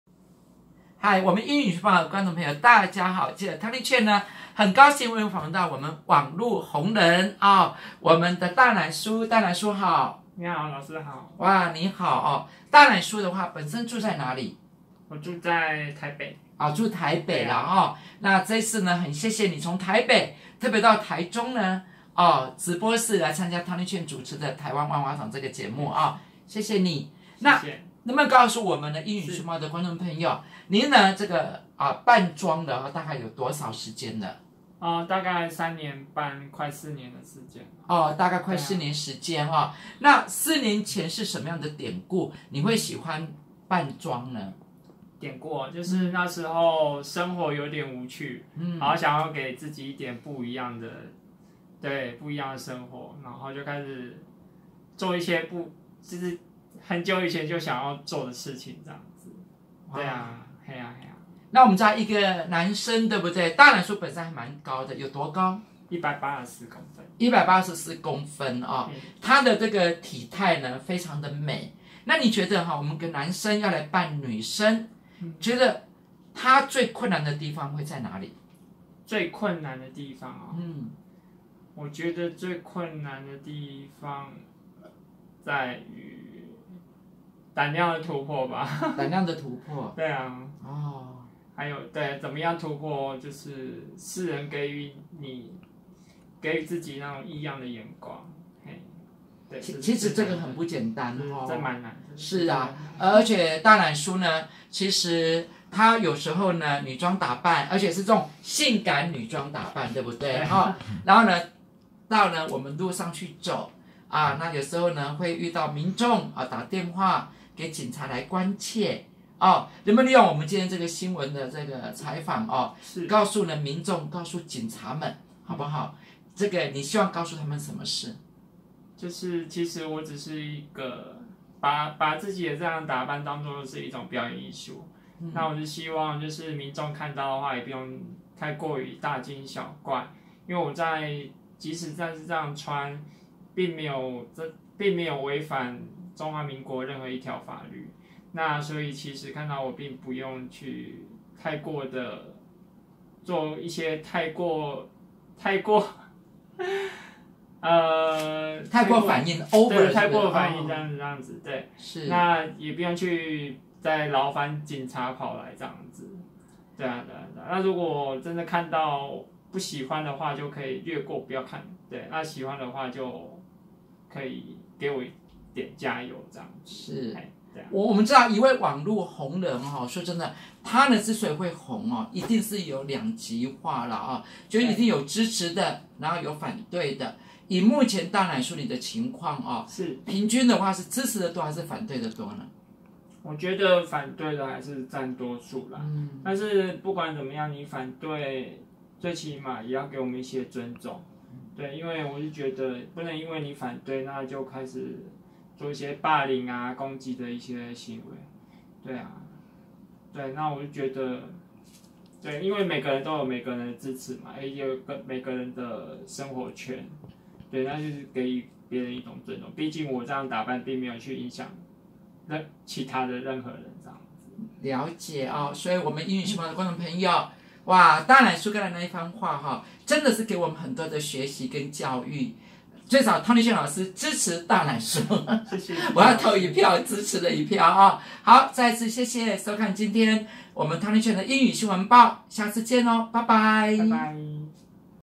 網紅大奶酥勇敢做自己，tonychen湯尼陳主持up直播節目，觀眾熱情，給予鼓勵!
湯尼陳在Up直播節目每天中午12點台灣萬花筒每天晚上8點湯尼陳教英語，另外兩個外景節目 好看趴趴走和湯尼陳送愛心，歡迎社會大眾主動推薦報名參加，報名方式LINE 8253tc或電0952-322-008。